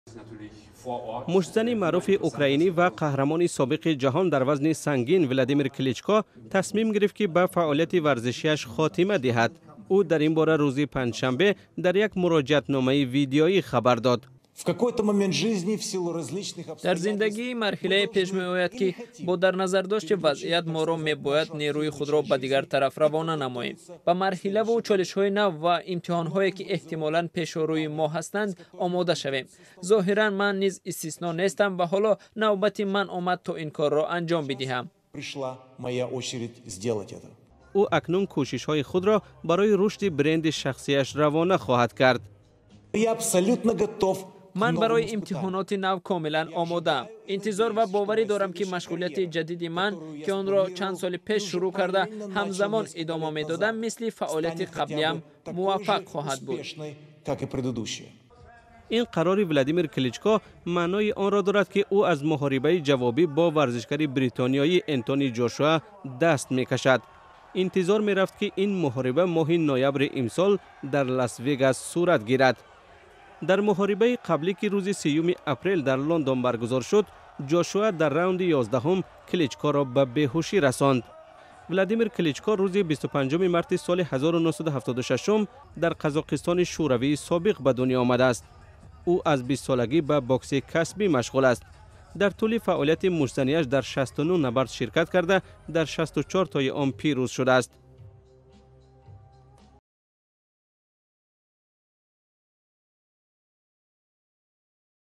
Владимир Кличко аз бокс рафт
Муштзани маъруфи украинӣ Владимир Кличко дар 41-солагӣ аз бокс рафт.